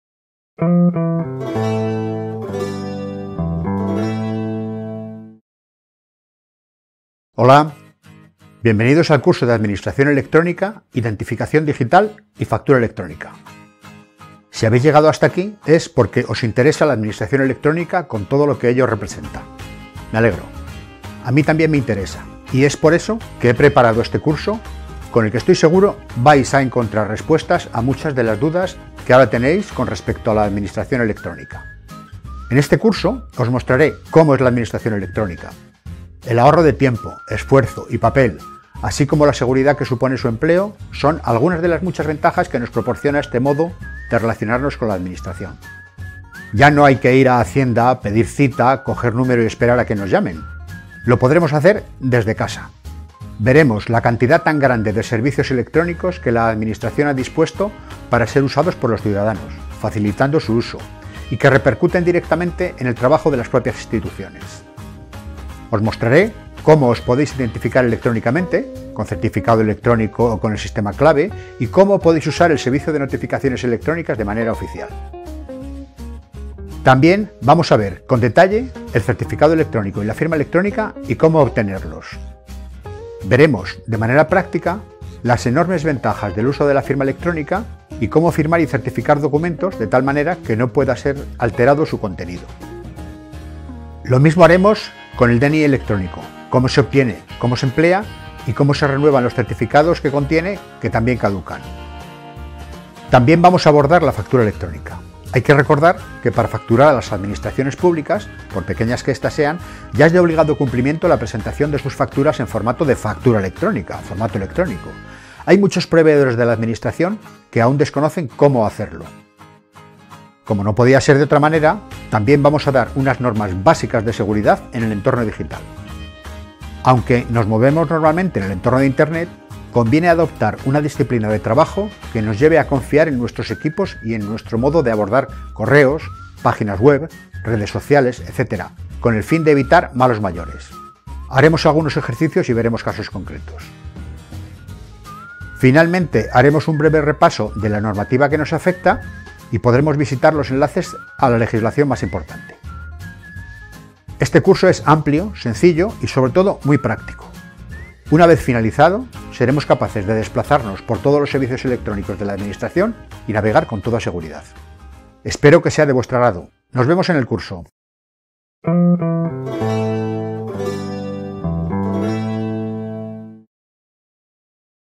Curso de Administración Electrónica. Identificación Digital y Factura Electrónica
En este Curso mostraremos los aspectos más importantes, desde el punto de vista práctico, del empleo de la identificación electrónica en nuestras relaciones con la Administración, a través de las distintas Sedes electrónicas que ésta pone a nuestra disposición para realizar los diferentes trámites electrónicos en los que en unos casos nos facilitan el trámite y en otros muchos estamos obligados.

Veremos los diferentes modos de identificarse electrónicamente, haciendo hincapié en el DNI electrónico y por lo tanto en los Certificados electrónicos y abordaremos algunas cuestiones básicas de seguridad en el entorno digital.

Como aplicación reina, veremos la Firma electrónica, sus tipos, usos y qué normativa la regula.

Un capítulo importante, dentro de la administración electrónica, merece la Factura electrónica, habida cuenta de la obligatoriedad de presentación en este formato, de las facturas generadas por los proveedores a  la Administración. En este curso hablaremos de la plataforma FACE y los códigos de las Unidades administrativas que intervienen en el proceso contable. Haremos un recorrido por el programa FacturaE.

Mencionaremos también el talante de la administración en cuanto a  las notificaciones electrónicas, que sustituyen a las entregadas personalmente por un agente, con todo lo que ello conlleva.

Hablaremos de las posibilidades, ventajas e inconvenientes del pago telemático.

Haremos un resumen de los servicios de la Administración electrónica y las diferentes aplicaciones que pone a nuestra disposición y mostraremos un compendio de la normativa más importante.